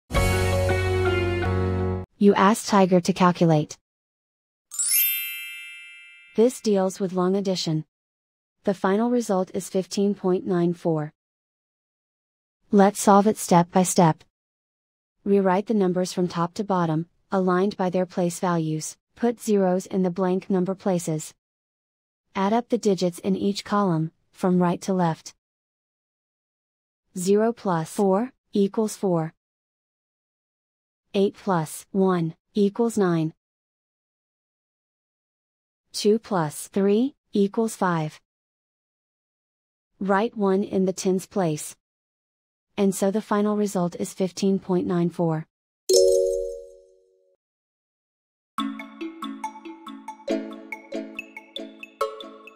Long addition: Solve 12.8+3.14 step-by-step solution
for a step-by-step and more tips and tricks for dealing with long addition.